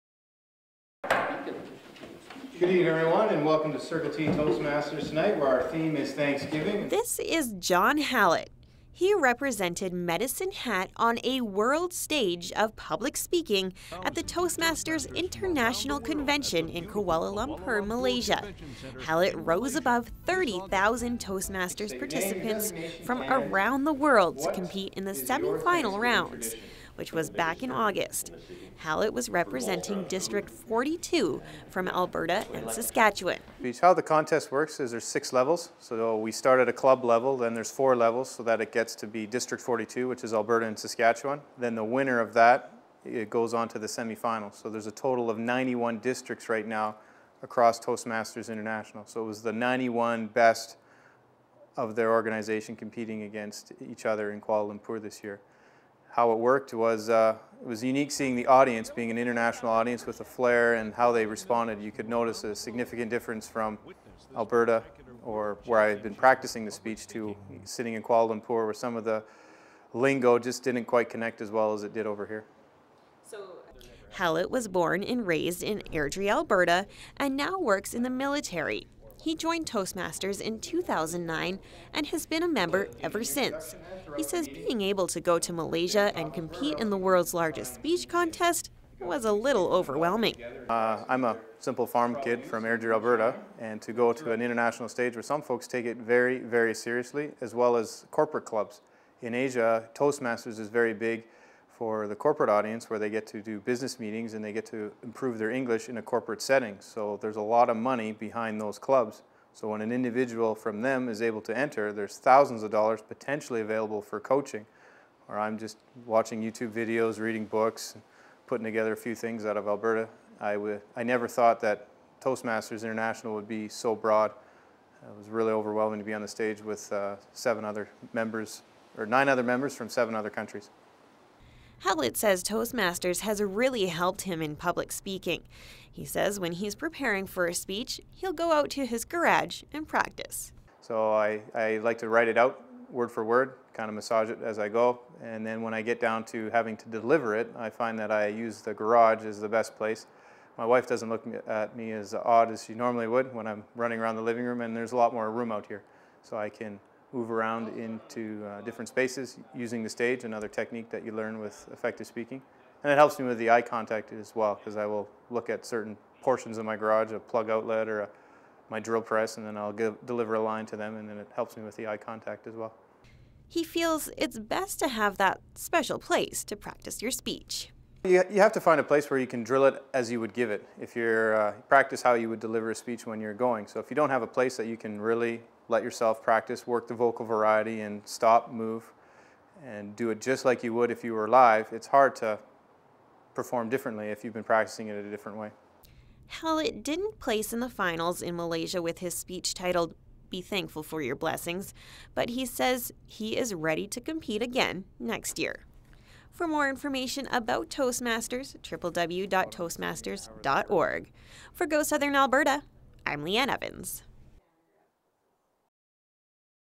John Hallett
Leanne Evans talks to Toastmasters participant John Hallett about his trip to the worlds largest speech contest.